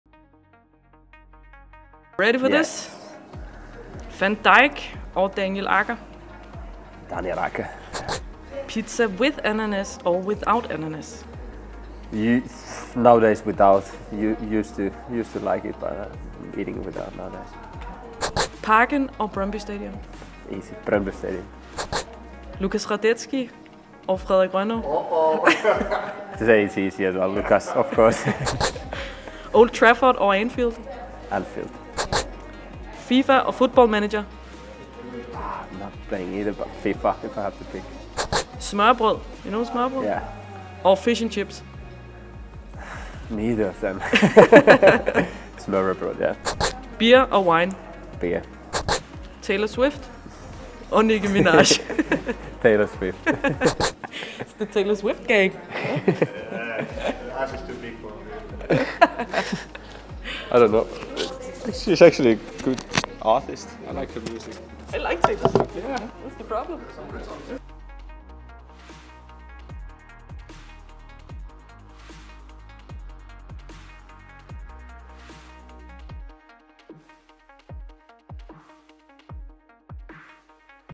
9 hurtige til Teemu Pukki
Thumbs up til Teemu Pukki for at forstå hvad ANANAS er, når nu Nanna ikke kan finde ud af at oversætte det til engelsk :)))